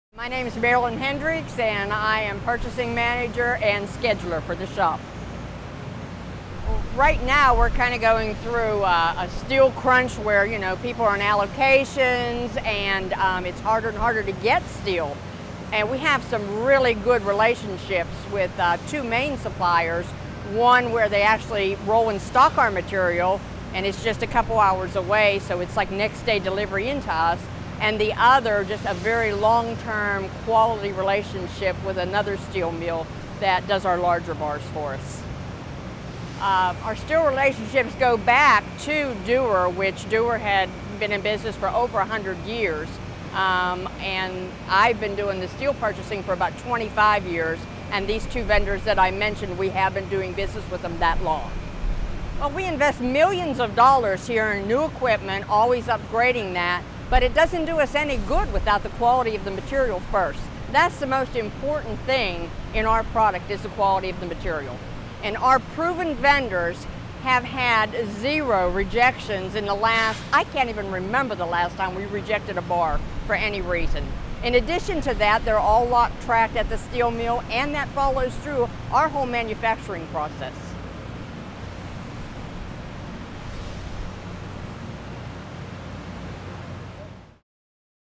Duer Carolina Coil Raw Materials for Hot Wound Coil Manufacturing
Duer Carolina Coil is a worldwide leader in manufacturing engineered hot wound coil springs for a variety of industrial applications. In this video, purchasing manager Marilyn Hendricks discusses the benefits of working with the highest quality raw materials.